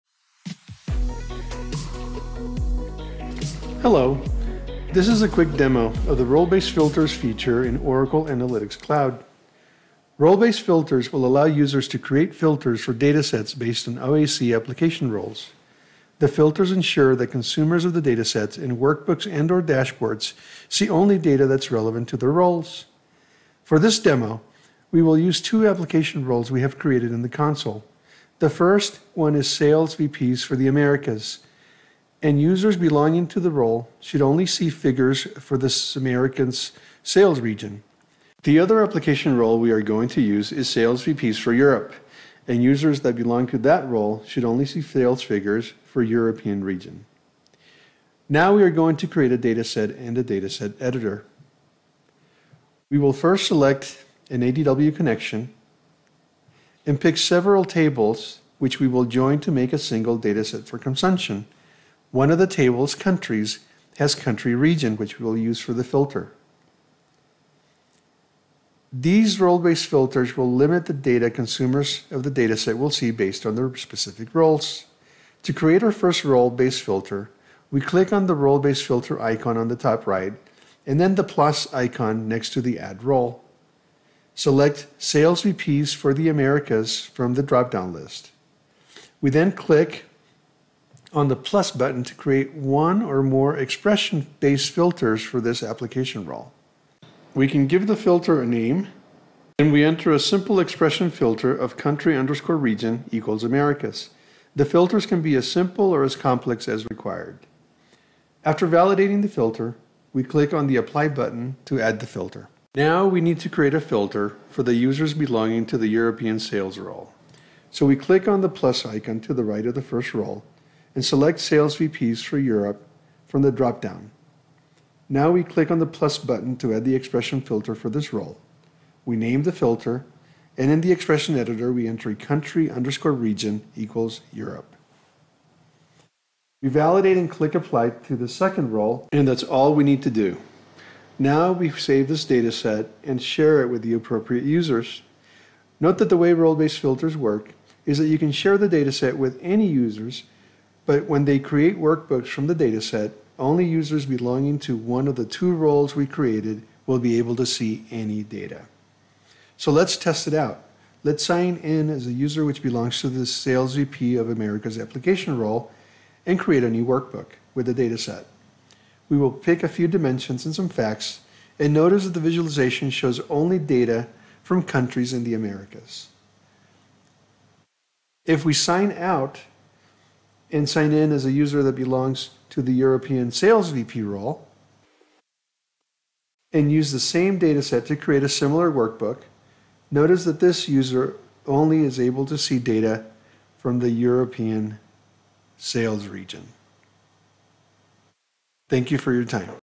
Applying Role Based Filter in Oracle Analytics Datasets
This video shows how to configure role-based filters directly on Oracle Analytics datasets, so that users belonging to a specific role only see a portion of the dataset data. This capability is part of the Oracle Analytics May 2023 update. A playlist of more of the new features coming with May 2023 update is available